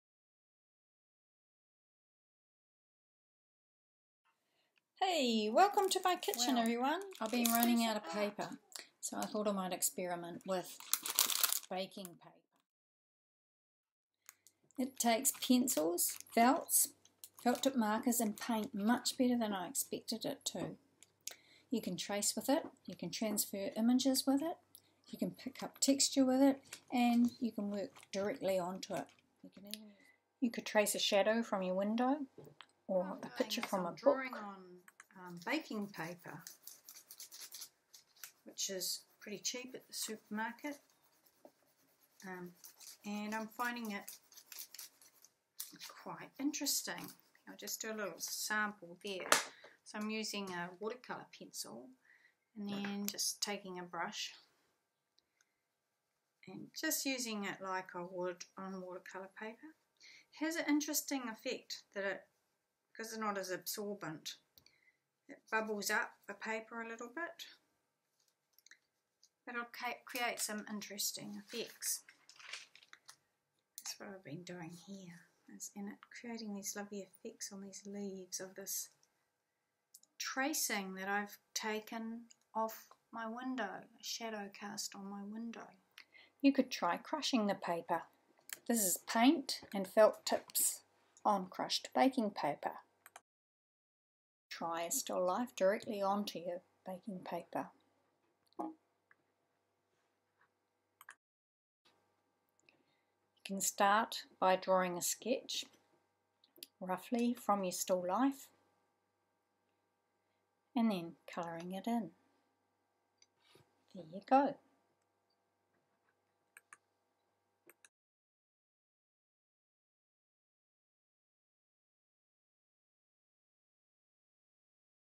Using Baking paper to draw on
How to create art in these Covid times when you are short of resources..